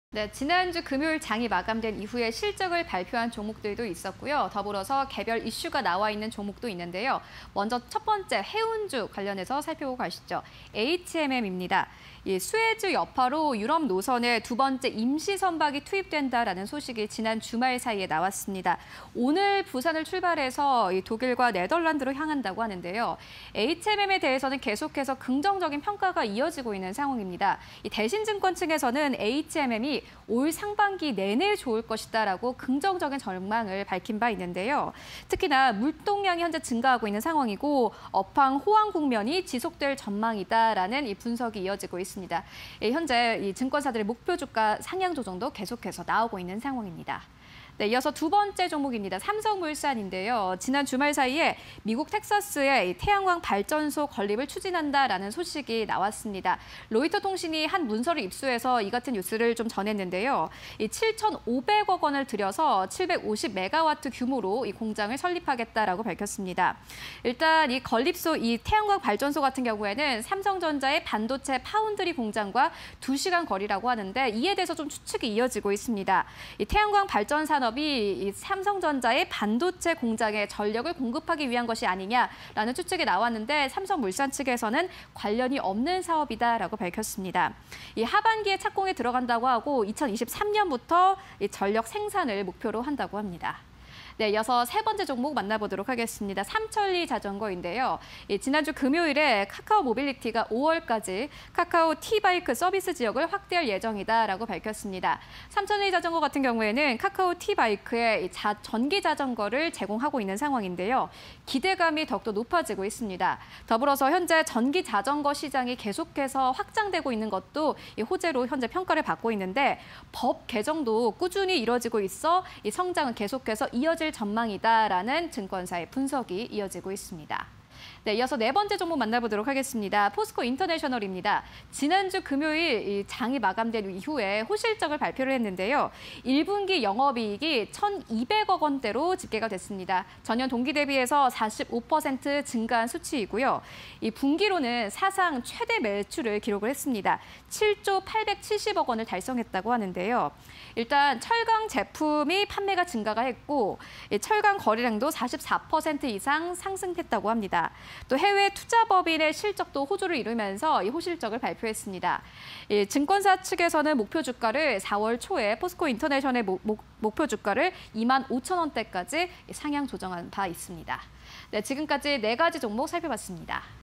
HMM, 삼성물산, 삼천리자전거, 포스코인터내셔널_공시&뉴스 (20210426)
- '수에즈 여파' 유럽노선에 두 번째 임시 선박 투입: 26일 부산 출발해 네덜란드·독일로
- 대신證 “HMM, 올 상반기 지속적으로 좋을 것”
- 물동량 증가, 업황 호황국면 지속 전망

- 탈석탄 본격화… 美텍사스 태양광발전소 건립 추진: 7,500억원 들여 700㎿급 규모: 삼성전자 반도체 파운드리 공장 2시간 거리
- 하반기 착공, 2023년 전력 생산

- 카카오모빌리티, 5월까지 '카카오T바이크' 서비스 지역 확대 예정: 업무협약 체결… 카카오T바이크 전기자전거 제공
- 전기자전거 시장 확대 호재
- 법 개정도 꾸준히 이뤄지고 있어 성장 지속 전망

- 1분기 영업이익 1,269억… 전년대비 45.4%↑
- 분기 사상 최대 매출… 7조 870억원 달성
- 트레이딩 및 해외 투자법인 실적 호조
- 철강제품 판매 증가… 철강 거래량 44%↑